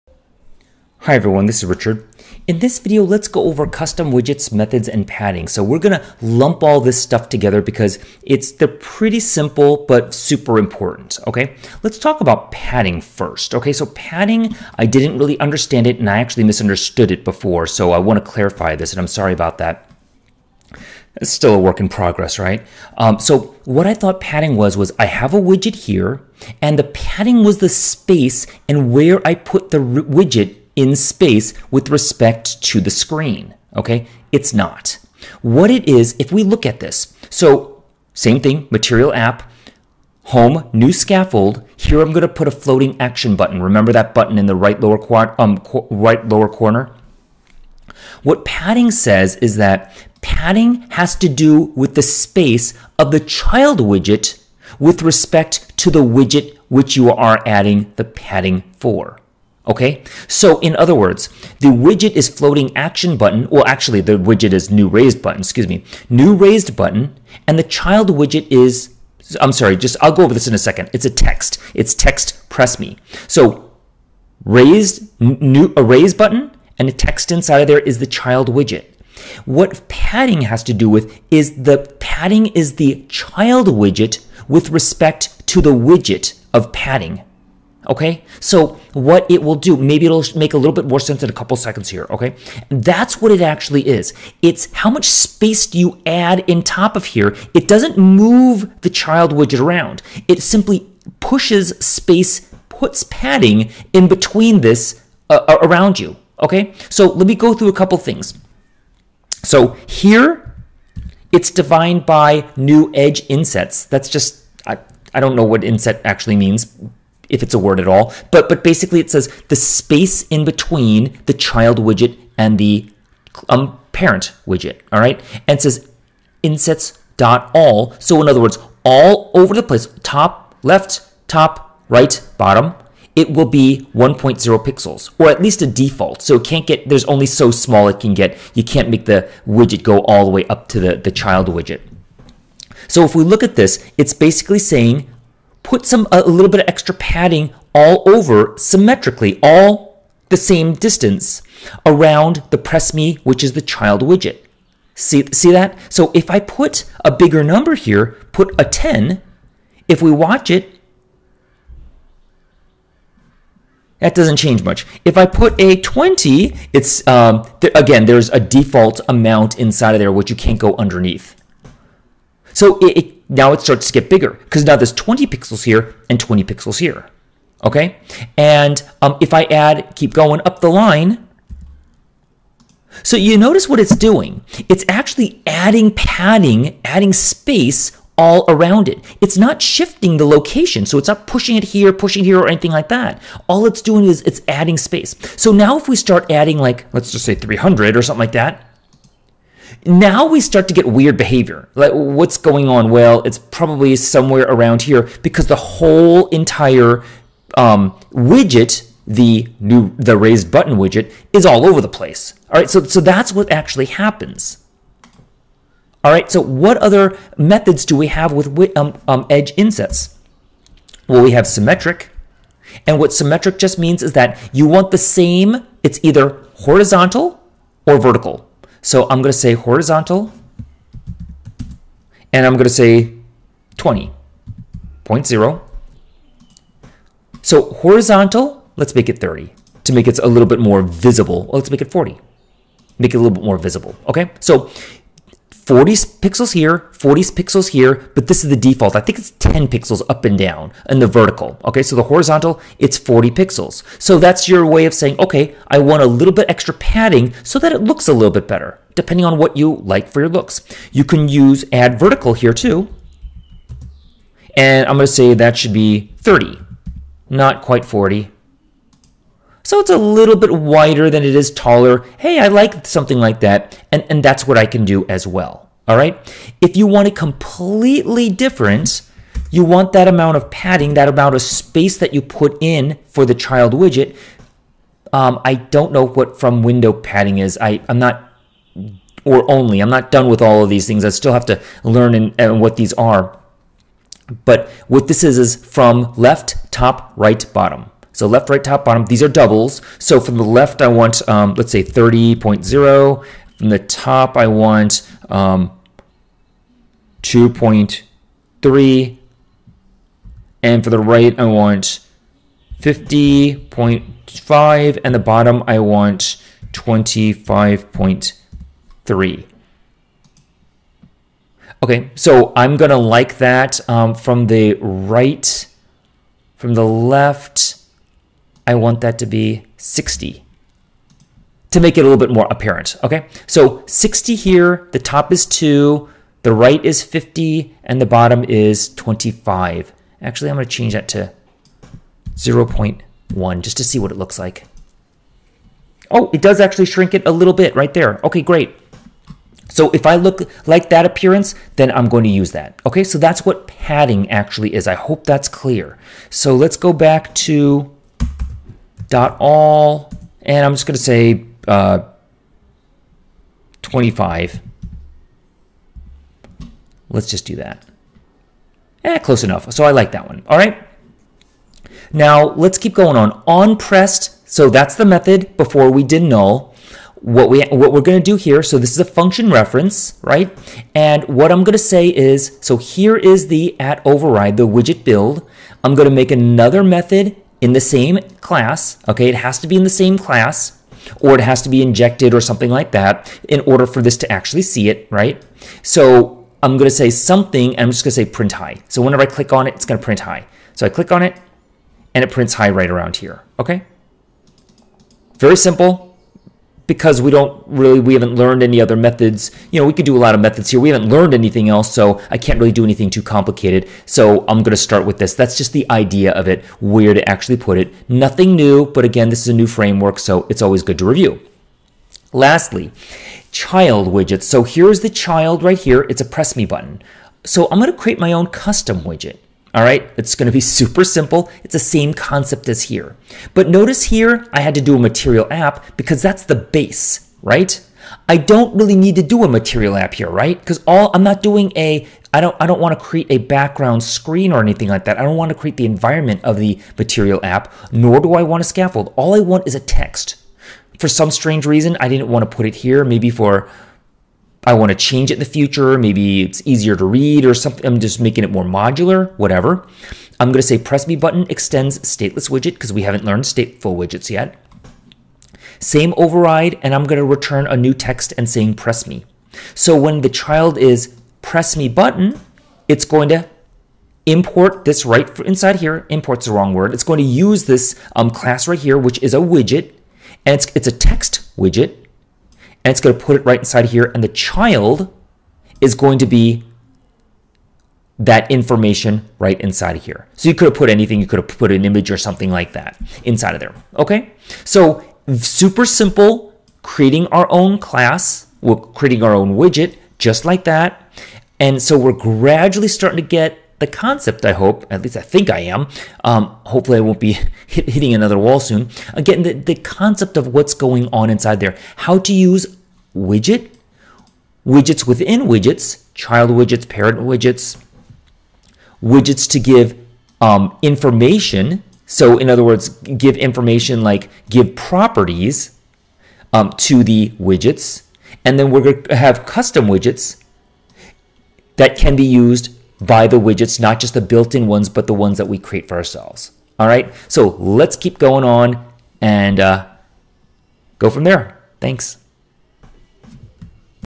Beginning Flutter: 11 Padding, Methods, and More Widgets
In this video, we review the following concepts:
padding - determines how much space the child widget should have added to make the edge of the parent widget.  It does not directly determine the location of the child widget.  It only adds space to define borders better.
methods/functions - must be in the same class as the widget.  Otherwise, must be instantiated or injected into that class.
Custom widgets - additional widgets do not need the structural widgets as the root widget does.  For now, they just need the same structure as the Stateless Widgets.